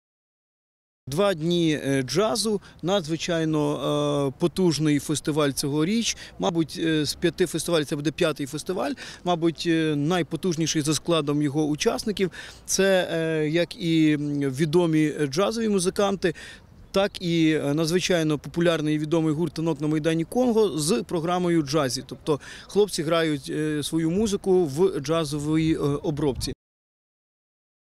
5-й «ДжазФест Поділля» відбудеться у Хмельницькому 11 та 12 травня
5-й «ДжазФест Поділля» відбудеться у Хмельницькому 11 та 12 травня. Цьогорічне дійство відбуватиметься на двох локаціях: на майданчику біля кінотеатру імені Тараса Шевченка та майданчику поблизу торгового центру «Дитячий світ».